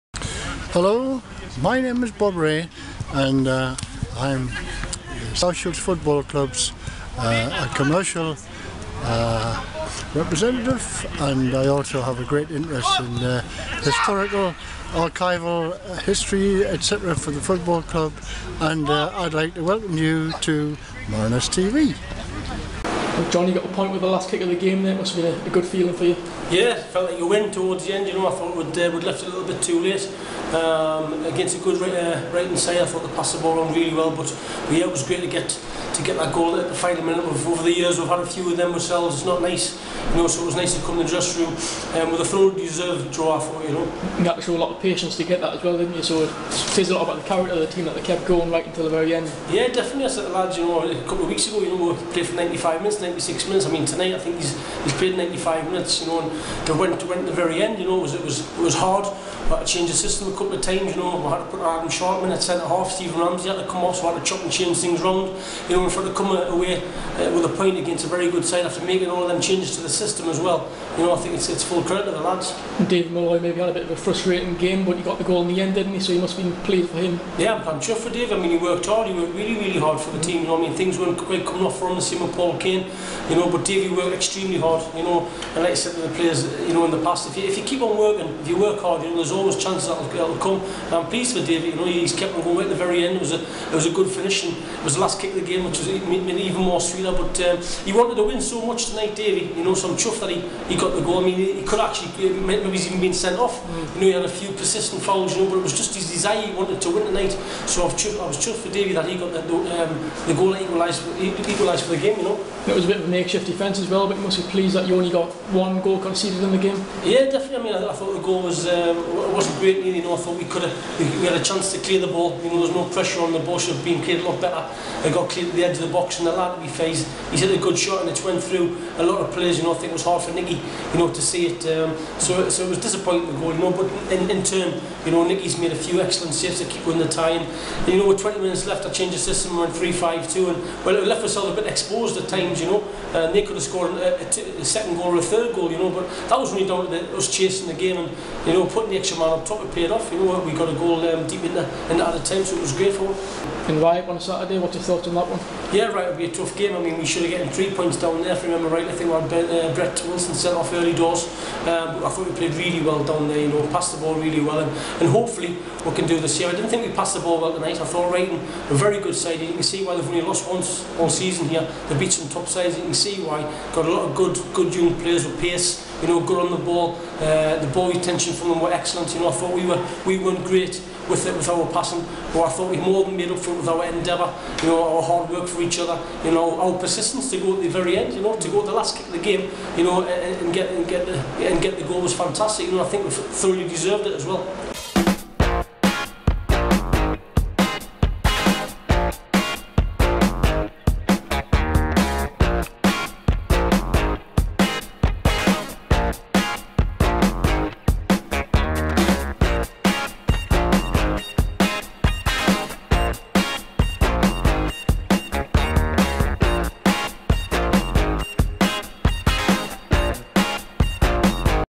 King: Ryton draw felt like a win
South Shields manager Jon King gives his reaction after the 1-1 draw at Ryton and Crawcrook Albion in Ebac Northern League Division Two. He praises David Molloy, who scored the last-gasp equaliser, as well as his side's character and persistence.